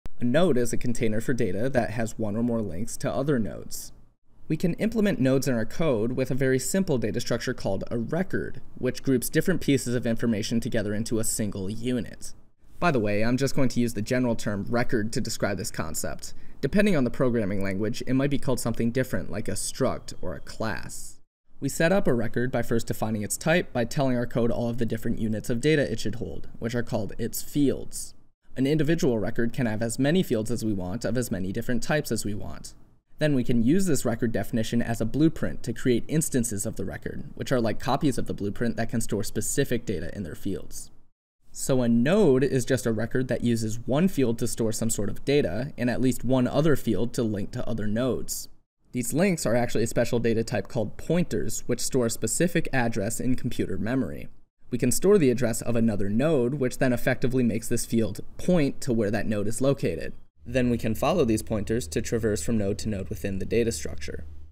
Linked Nodes Data Structures in one minute - everything you need to know!
Linked Nodes are a fundamental data structure where individual Nodes contain data and link to other Nodes using Pointers